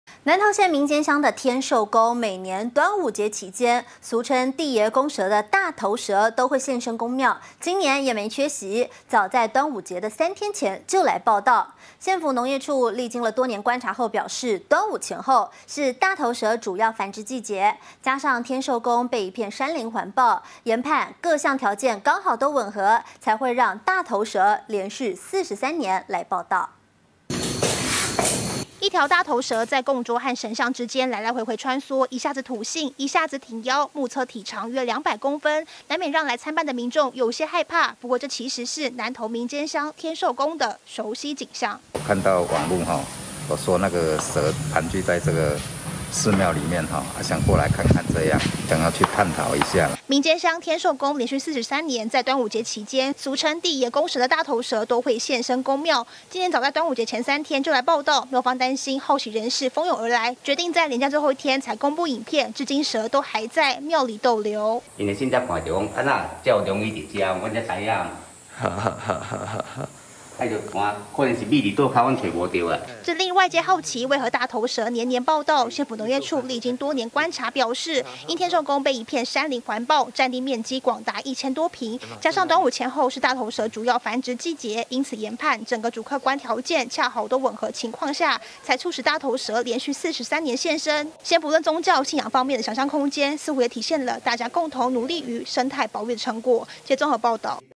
連43年端午節報到!帝爺公蛇現身天受宮
南投縣名間鄉天受宮，每年端午節期間，俗稱「帝爺公蛇」的大頭蛇都會現身宮廟，今年也沒缺席，早在端午節前3天就來報到，縣府農業處歷經多年觀察表示，端午前後，都是大頭蛇主要繁殖季節，加上天受宮被一片山林環抱，研判因各項條件恰好吻合，才使大頭蛇連續43年來造訪。